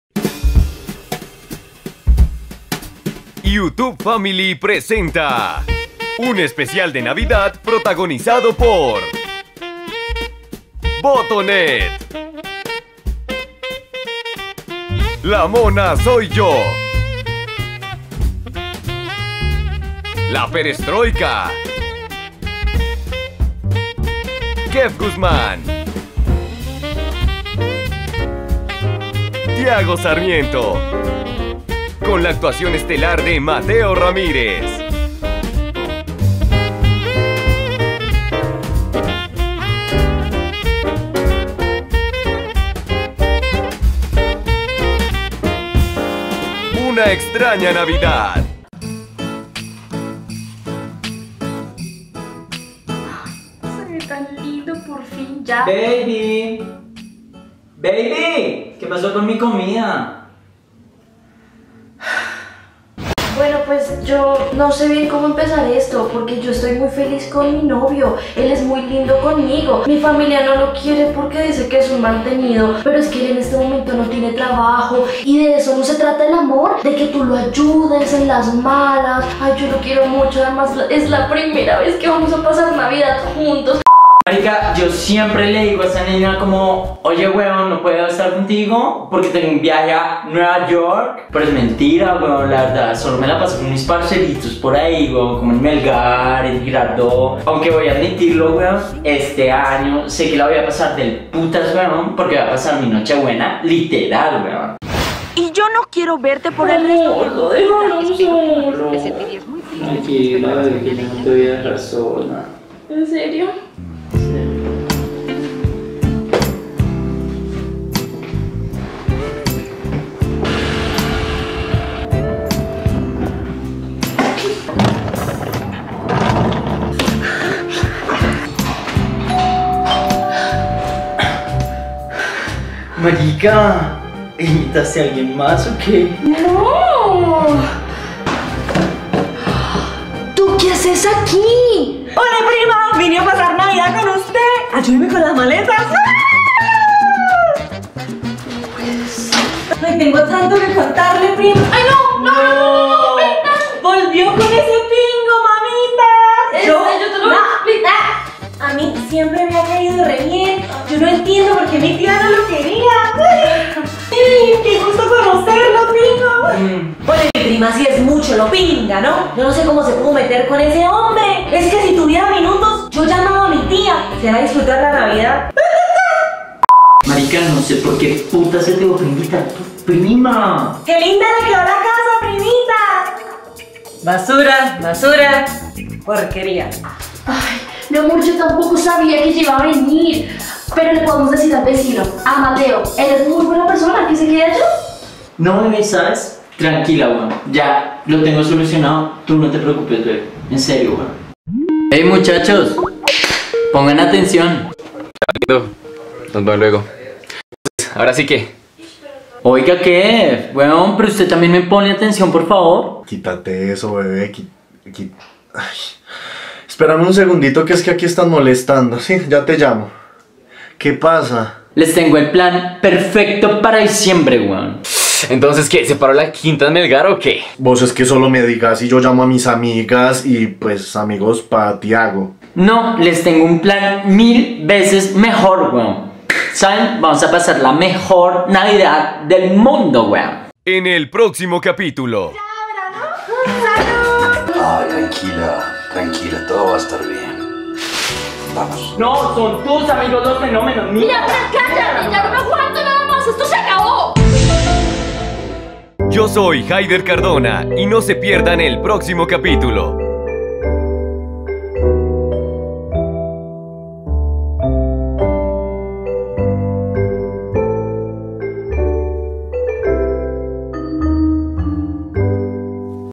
UNA EXTRAÑA NAVIDAD 1/6 | BOTONET
AMIGOOOS!! Este video es el primer capitulo de una mini serie web que hicimos junto a mis amigos La Mona Soy Yo, Keff Guzmán, Tiago Sarmiento, La Pereztroica y Mateo Ramirez.

Esoeramos les guste mushote! 😂😂😂

✩ Botonët Says Mëow ✩